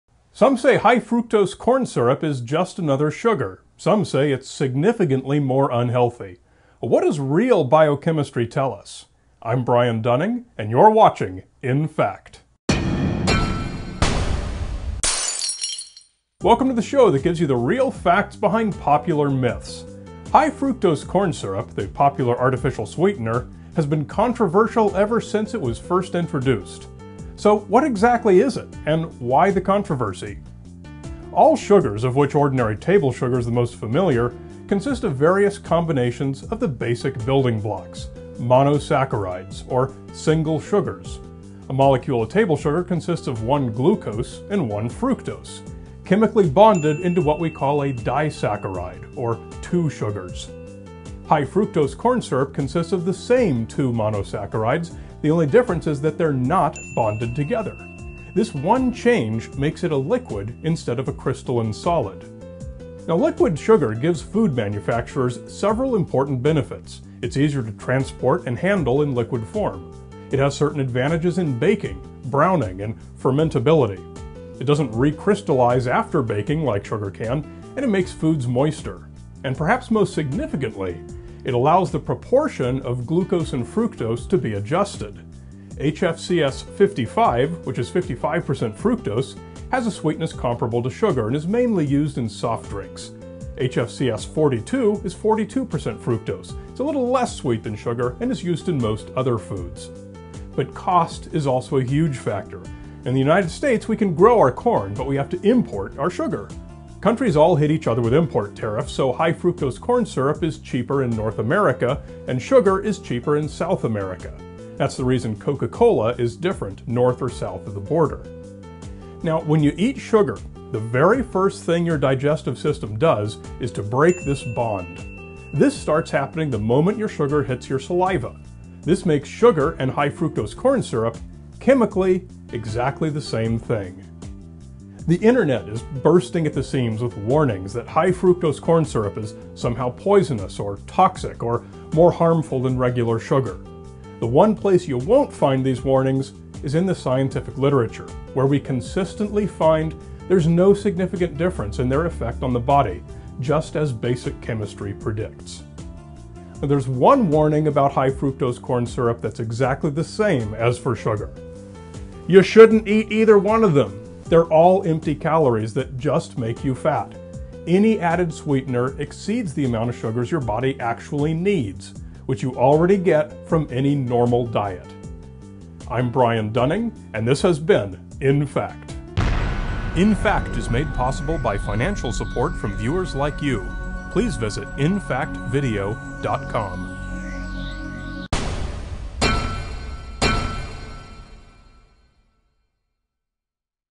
inFact: High Fructose Corn Syrup
Some say High Fructose Corn Syrup is just another sugar; some say it's significantly more unhealthy. What does real biochemistry tell us?

High fructose corn syrup, the popular artificial sweetener, has been controversial ever since it was first introduced. So what exactly is it, and why the controversy?

All sugars, of which ordinary table sugar is the most familiar, consist of various combinations of the basic building blocks: monosaccharides, or single sugars. A molecule of table sugar consists of one glucose, and one fructose, chemically bonded into what we call a disaccharide, or two sugars.

High fructose corn syrup consists of the same two monosaccharides; the only difference is that they're not bonded together. This one change makes it a liquid instead of a crystalline solid.

Liquid sugar gives food manufacturers several important benefits. It's easier to transport and handle in liquid form. It has certain advantages in baking, browning, and fermentability. It doesn't recrystallize after baking like sugar can, and makes foods moister. And perhaps most significantly, it allows the proportion of glucose and fructose to be adjusted. HFCS 55, which is 55% fructose, has a sweetness comparable to sugar and is used mainly in soft drinks. HFCS 42 is 42% fructose, and is a little less sweet than sugar and is used in most other foods.

But cost is also a huge factor. In the United States, we can grow our corn; but we have to import our sugar. Countries all hit each other with import tariffs, so high fructose corn syrup is cheaper in North America, and sugar is cheaper in South America. That's the reason Coca-Cola is different north or south of the border.

Now when you eat sugar, the very first thing your digestive system does is to break this bond. This starts happening the moment the sugar hits your saliva. This makes sugar and high fructose corn syrup, chemically, exactly the same thing.

The Internet is bursting at the seams with warnings that high fructose corn syrup is somehow poisonous or toxic or more harmful than regular sugar. The one place you won't find these warnings is in the scientific literature, where we consistently find there's no significant difference in their effect on the body, just as basic chemistry predicts.

There's one warning about high fructose corn syrup that's exactly the same as for sugar: YOU SHOULDN'T EAT EITHER ONE OF THEM; they're all empty calories that just make you fat. Any added sweetener exceeds the amount of sugars your body actually needs, which you already get from any normal diet.